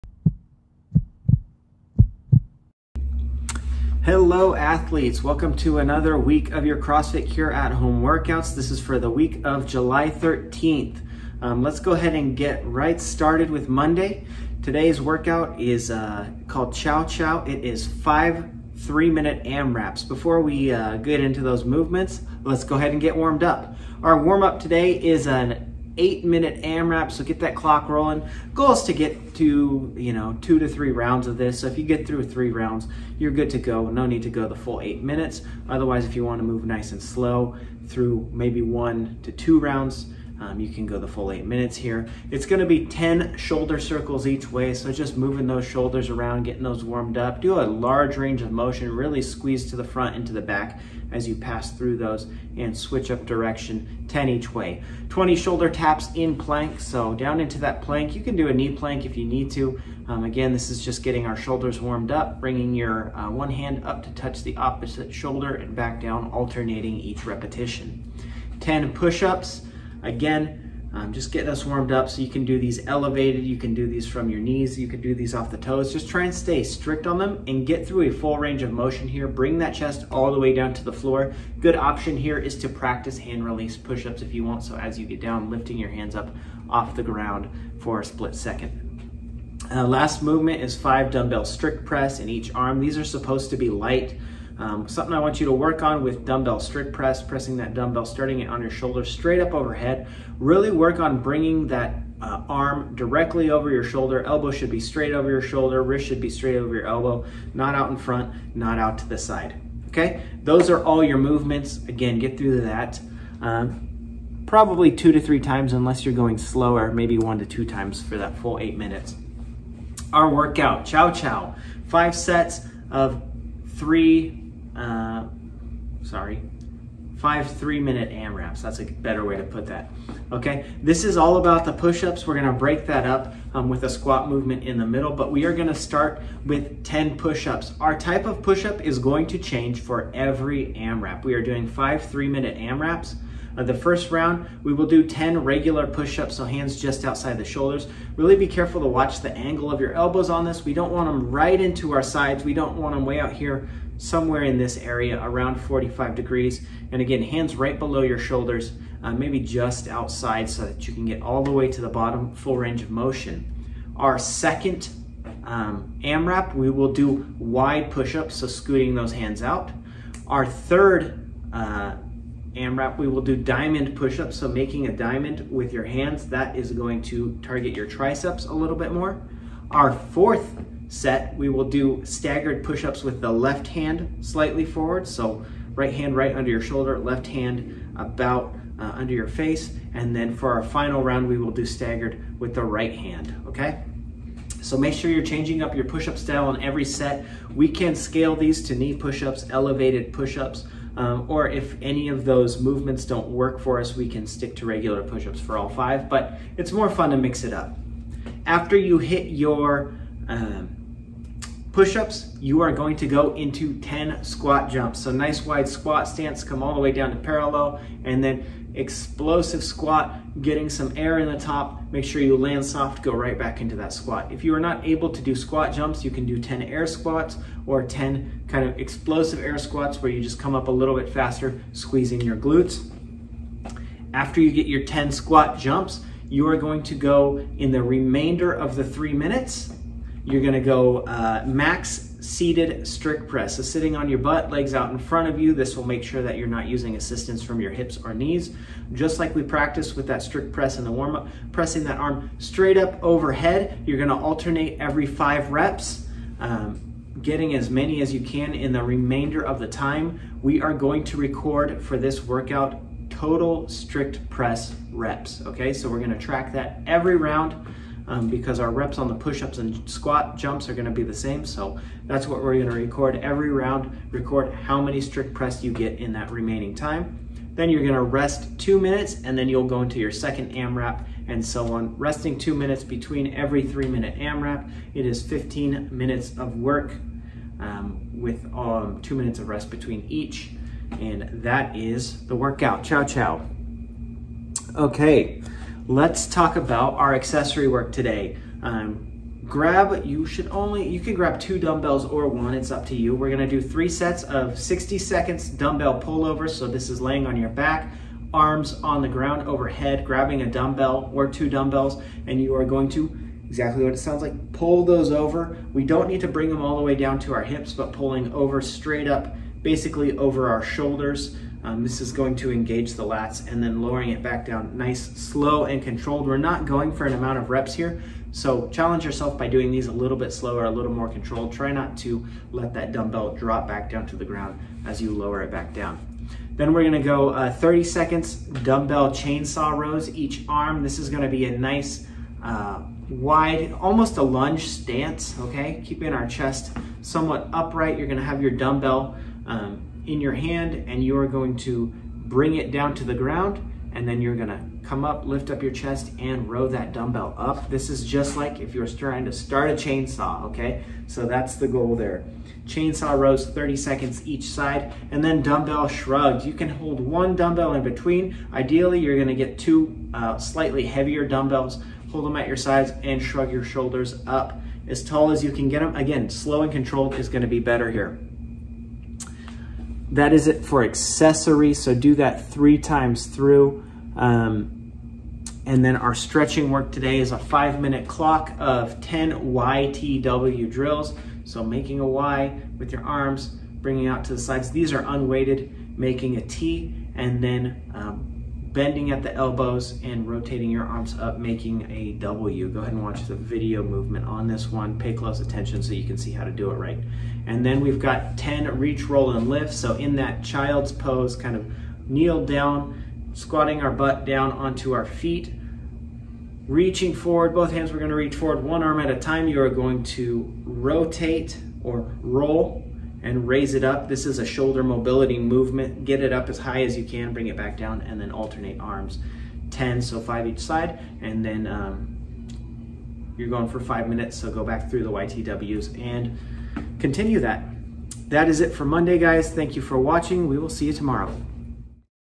MONDAY JULY 13th CrossFit Cure Home Workout "Chow Chow"
Warm Up:
**Warm-up AMRAP 8:00**
10 shoulder circle each way
20 Shoulder Taps in Plank Position
10 Push-ups (from toes or knees)
5 DB Strict Press / arm (light)

“Chow Chow”
5 Sets:
AMRAP 3:
10 Pushup (regular, wide, diamond, staggered L, staggered, R)
10 Squat Jump
Max (alternate every 5) seated strict press w/ time remaining.
2:00 rest btw. sets.

Accessory:
3 Sets:
:60 DB Pull Overs
:30 DB Chainsaw Rows / Arm
:60 DB Shrugs

Bodyweight Option:
3 Sets:
:60 Shoulder Taps
:30 Split Squats / Side
:60 Flutter Kicks

Stretching:
On a 5:00 Clock:
10 Y-T-W Drills
10 Reach-Roll-Lift